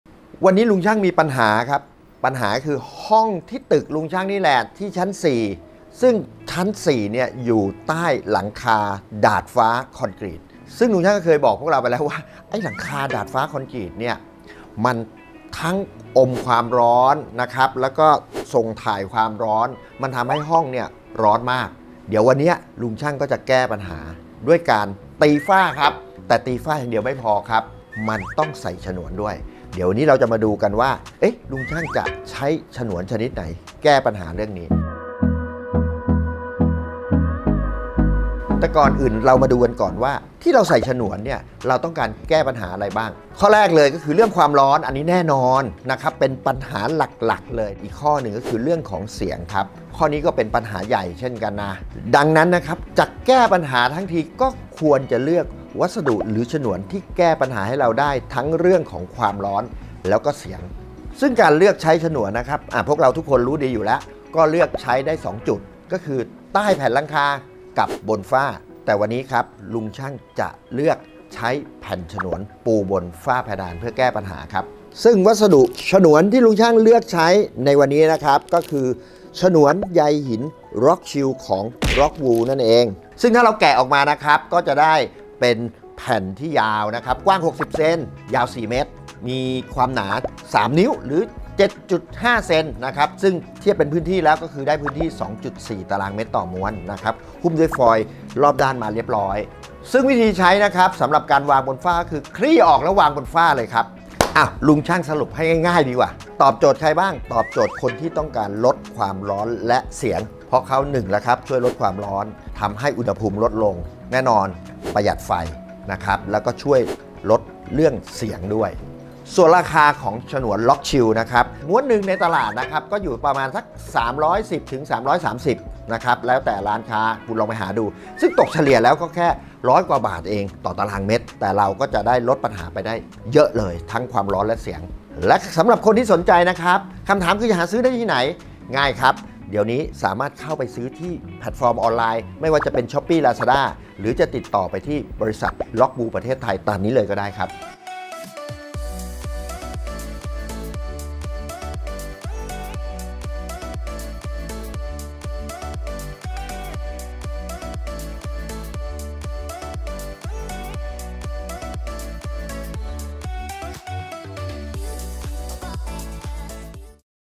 บ้านร้อนแล้วไง ? แก้ไขด้วย... | คุยกับลุงช่าง
อากาศเมืองไทยที่มีแต่ร้อน และร้อนมาก จะหลบแดดคลายร้อนในบ้านก็ยังไม่หาย ทั้ง ๆ ที่ก็มีหลังคากันแดดแล้วแท้ ๆ แต่เนื่องจากรังสีความร้อนจากดวงอาทิตย์ได้แผ่ผ่านหลังคาเข้ามาในบ้าน ความร้อนก็จะยิ่งสะสมมากขึ้นเรื่อย ๆ
    วันนี้ลุงช่างมีเคล็ดลับที่ทำให้บ้านเย็นได้โดยไม่จำเป็นต้องพึ่งพาเครื่องปรับอากาศตลอดทั้งวันมาบอกทุกคน ซึ่งตัวช่วยนั้นก็คือ “ฉนวนกันความร้อนบนฝ้าเพดาน จาก ROCKWOOL รุ่น Rock Chill” นั่นเอง
ฉนวนวางบนฝ้า Rock Chill สามารถกันร้อนได้ถึง 6 เท่า หรือลดอุณหภูมิมากกว่า 3 องศาเซลเซียส และยัง สามารถกันเสียงรบกวนได้อีกด้วย